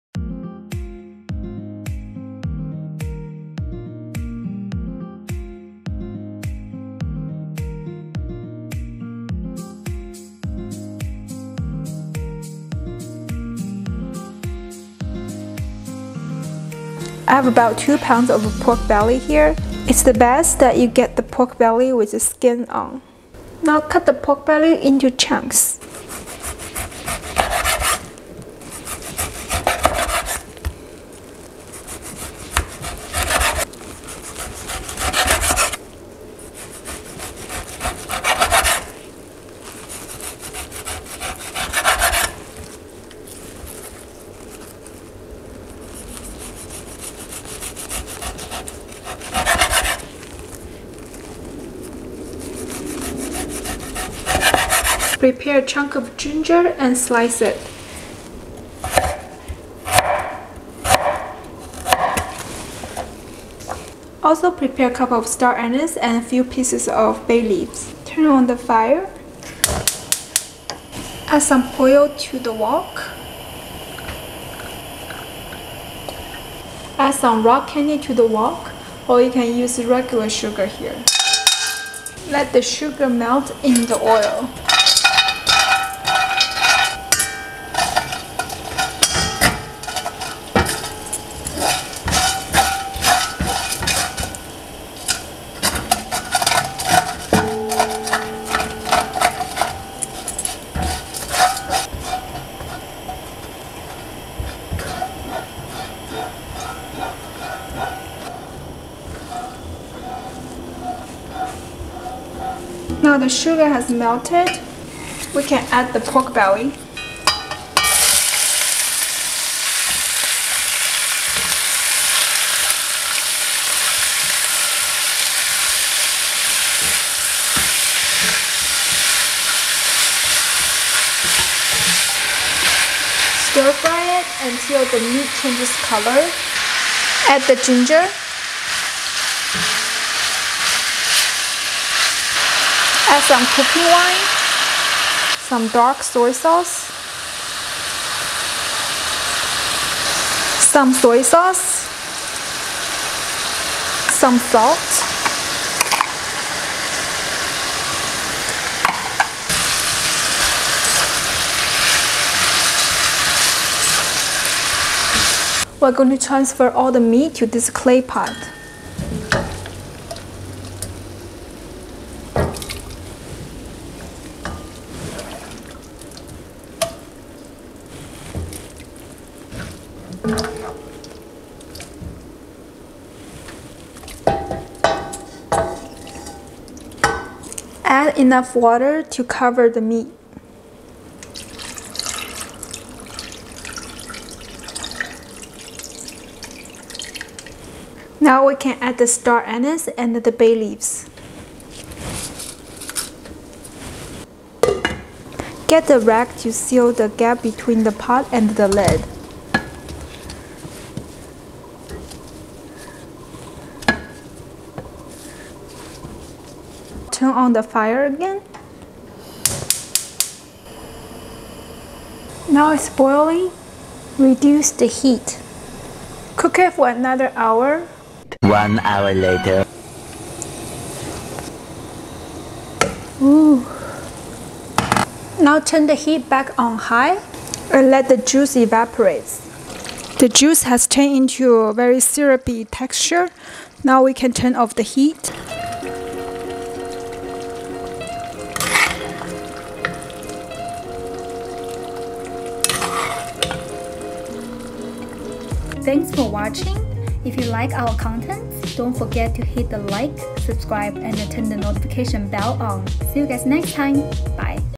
Homemade Braised Pork Belly Recipe (ASMR Cooking) | 如何在家做出软糯且不腻的红烧肉
Hope you find this homemade Braised Pork Belly recipe helpful. Enjoy it with your friends and families!

Ingredients:
2 lb. pork belly with skin on
1 tablespoon oil
2 teaspoon rock sugar or regular sugar
1/2 teaspoon dark soy sauce
1 1/4 teaspoon salt
2 tablespoon cooking wine
2 star anise
5-6 pieces bay leaves